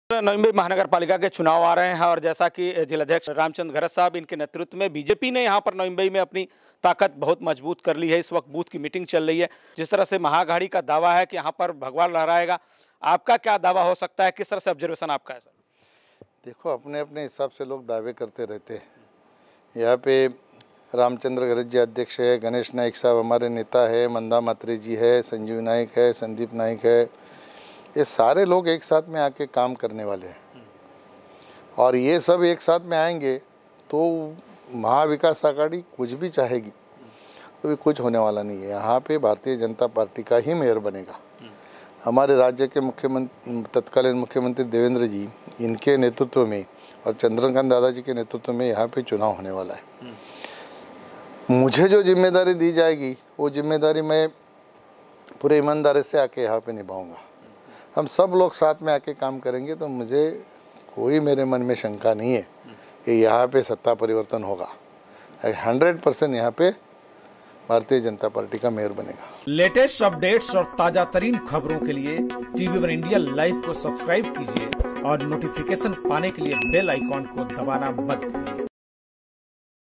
केंद्रीय मंत्री Kapil Patil: MVA लाख चाहे, नवी मुंबई में जीतेगी बीजेपी, बनाएगी मेयर
केंद्रीय मंत्री KapilPatil: MVA कितना भी प्रयास कर ले, NMMC में बनेगा BJP का मेयर